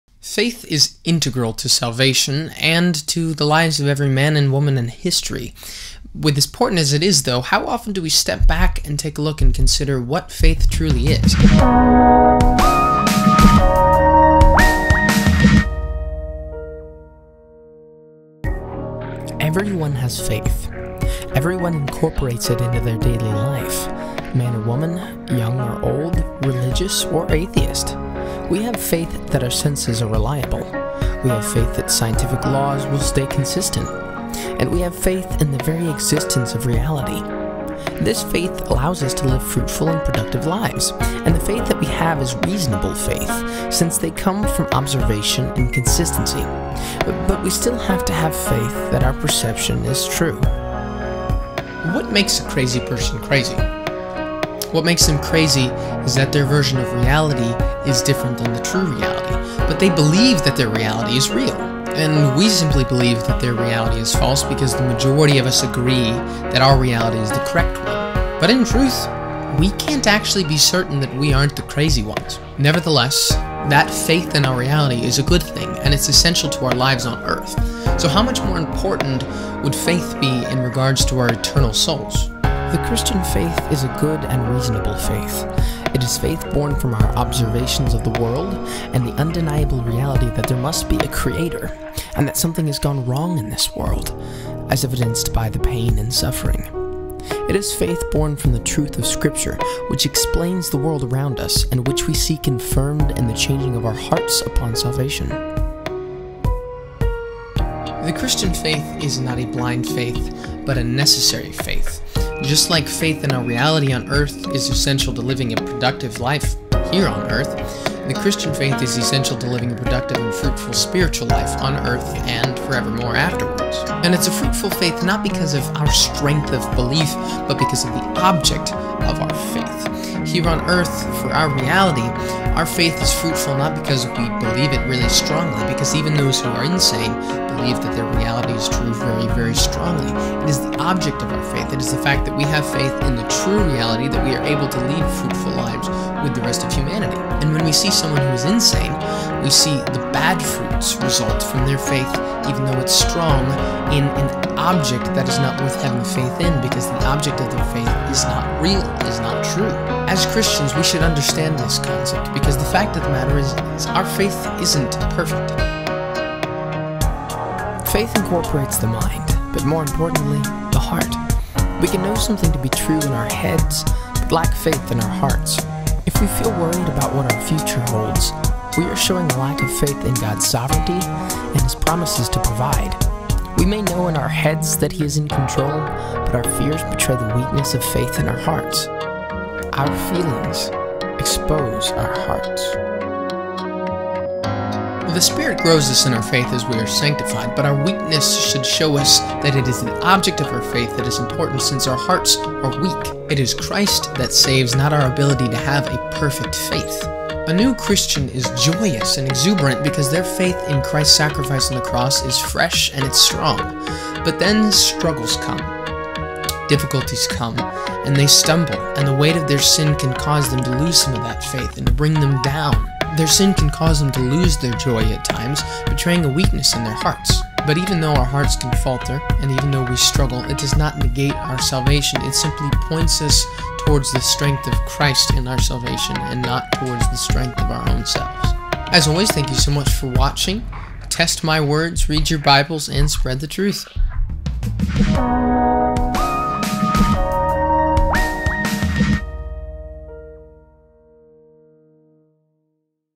What is Faith?
Faith is central to salvation, but what exactly does it mean to have faith?